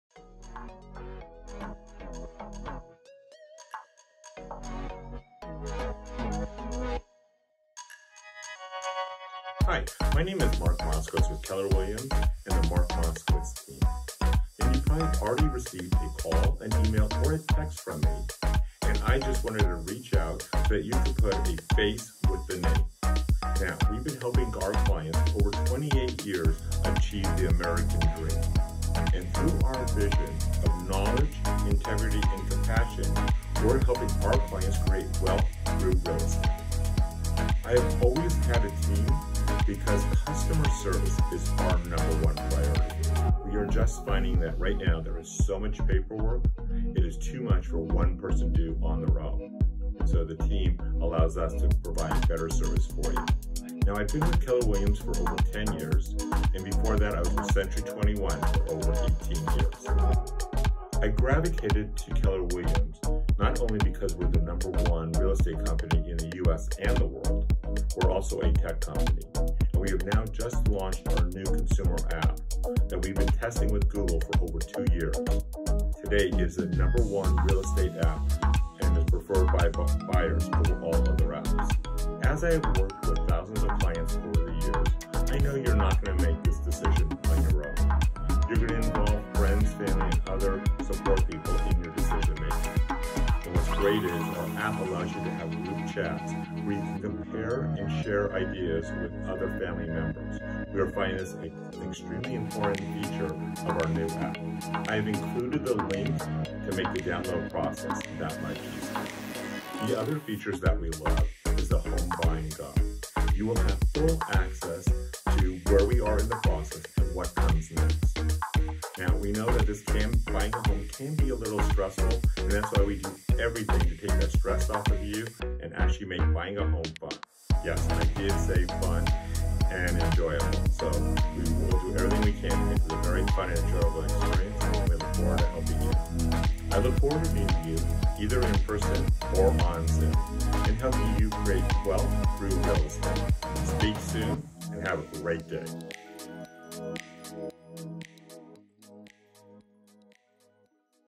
Buyers Guide
Mark takes you through the buying guide as you prep to purchase your new home!